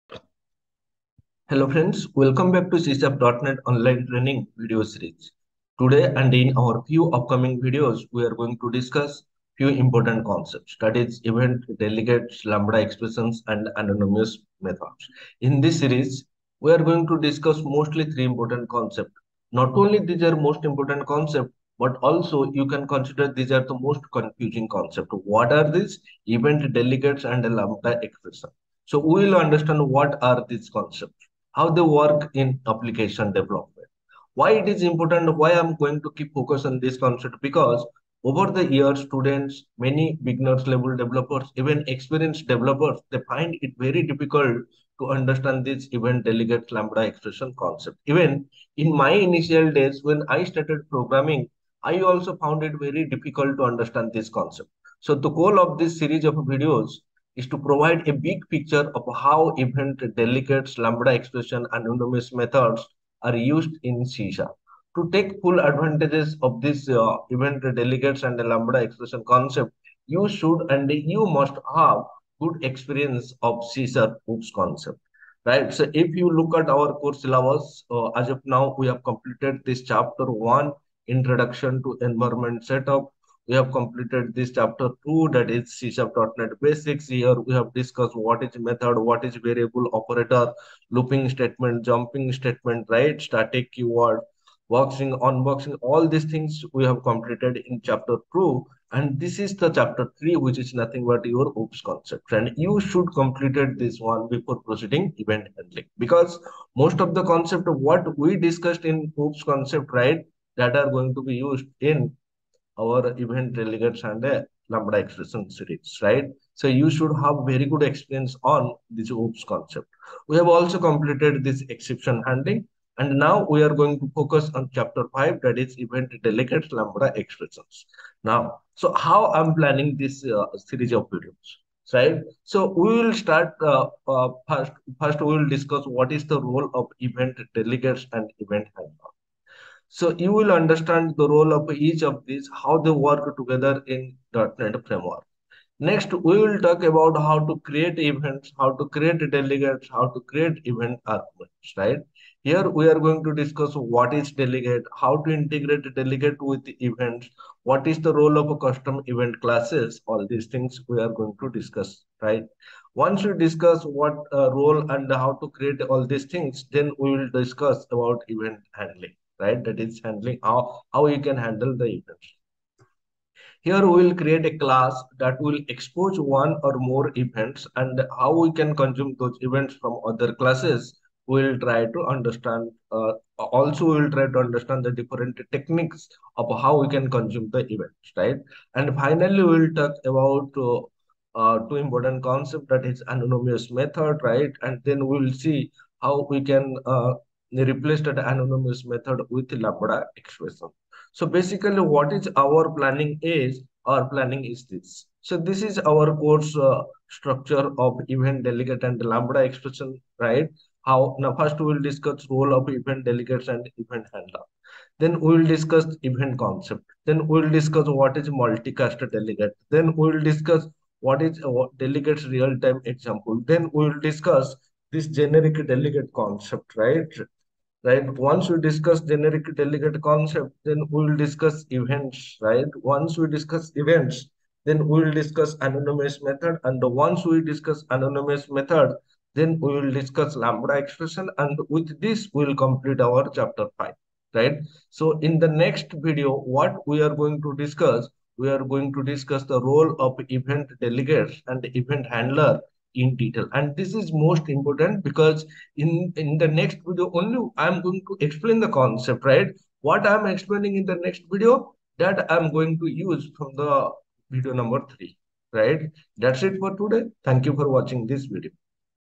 Course Structure of Events, Delegates, and Lambda Expression
Course Structure of Events, Delegates, and Lambda Expression in C#:
In this video series, we are going to discuss three important concepts and also most confusing concepts i.e. Events, Delegates and Lambda Expressions in C# and how they work in Application Development. Over the years students and many beginners level developers and even experience developers find it very difficult to understand these concepts. Even in my initial days, I also found it very difficult to understand these concepts. So, the goal of this series of videos is to provide a big picture of how events, delegates, and lambda expressions are used in C#. To take the full advantage of Events, Delegates, and Lambda Expression concepts, you should and must have good experience of C# OOPs concepts.

How we are going to cover the concepts?
1. First, we will discuss the Role of Events, Delegates, and Event Handlers. You need to understand the role of each one of these and how they work together in the .NET Framework.
2. Next, we will talk about How to Create Events, Delegates, And EventArgs in C#. Here, we are going to discuss what is delegate and how to integrate delegates with events, and what is the role of custom event classes.
3. Next, we will talk about Handling Events. Here, we will create a class that will expose one or more events and how we can consume those events from other classes and we will try to understand different techniques to consume the events.
4. Finally, we will talk about Lambda Expression and Anonymous Methods.

Course Structure of Events in C#
Course Structure of Delegates in C#
Course Structure of Lambda Expression in C#